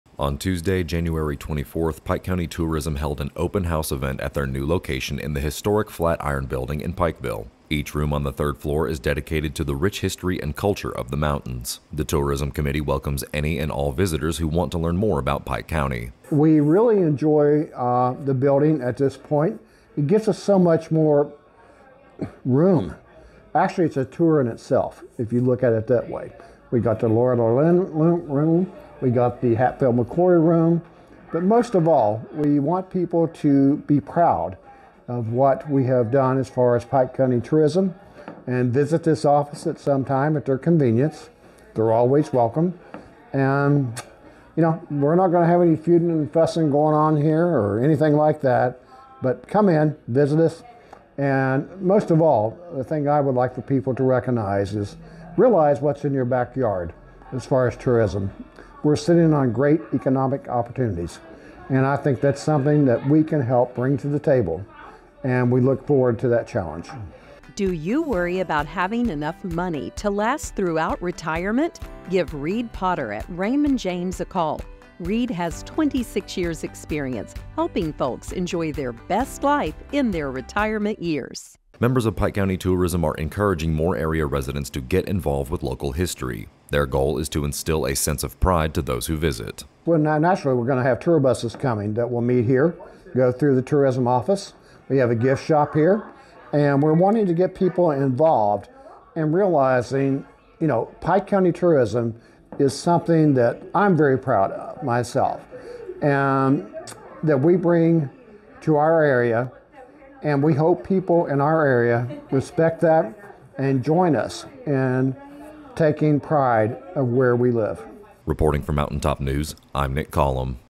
Pikeville and Pike County Welcome Center has a new Home
The Pikeville and Pike County Welcome Center has been moved to a historic landmark.